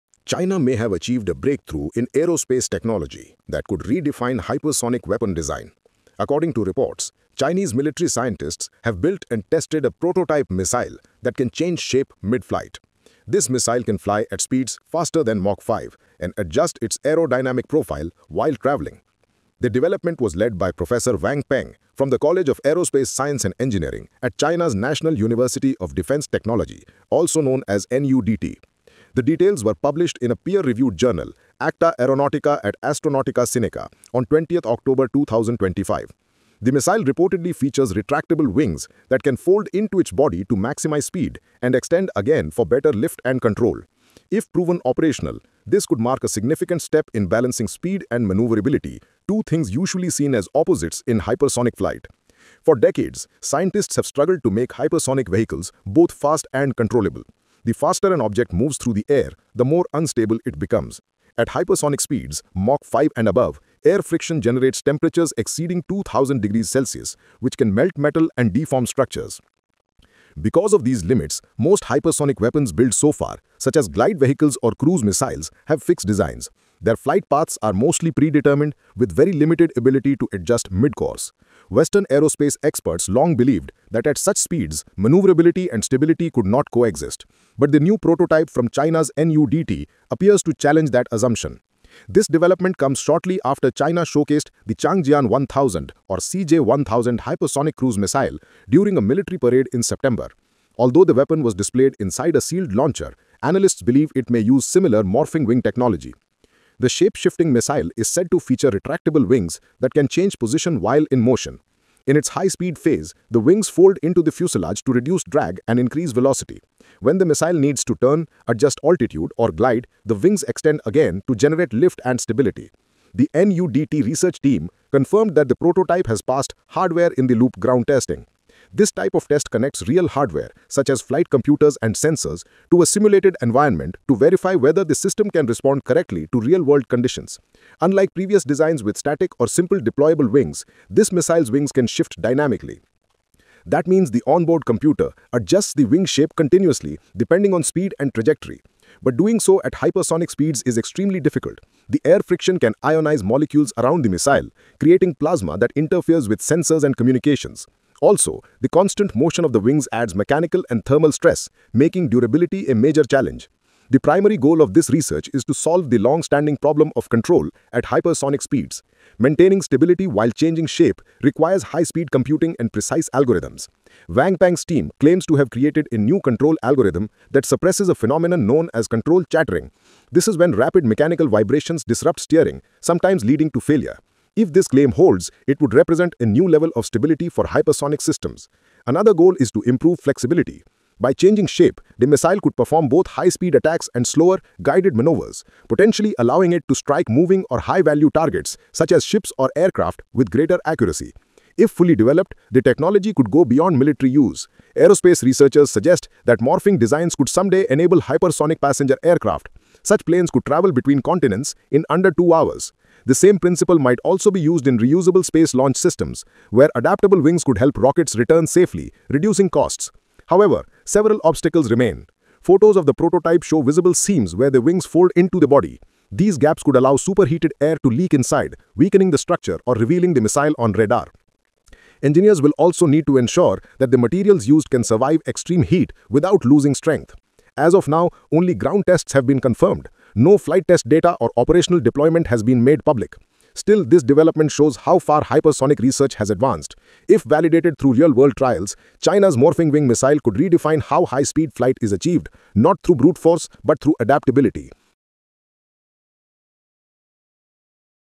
This Chinese Hypersonic Missile Can Shapeshift To Confuse Its Enemies
China has reportedly built a new hypersonic missile that can change shape mid-flight while travelling at speeds faster than Mach 5. The project, led by scientists at the National University of Defence Technology, introduces retractable wings that fold into the body for high-speed flight and extend again for lift and control.

The prototype has passed ground tests using a hardware-in-the-loop system, confirming that its design and algorithms can work in real conditions. This development aims to combine speed with manoeuvrability, a challenge that has limited hypersonic technology for decades.

If fully developed, such morphing-wing systems could influence both defence and civil aviation, making faster, more adaptable flight possible. However, technical challenges remain, including heat resistance, plasma interference, and structural durability.